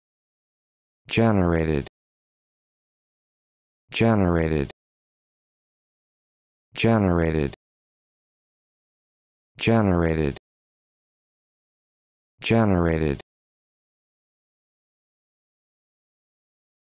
How to Pronounce Generated
This video shows you how to pronounce Generated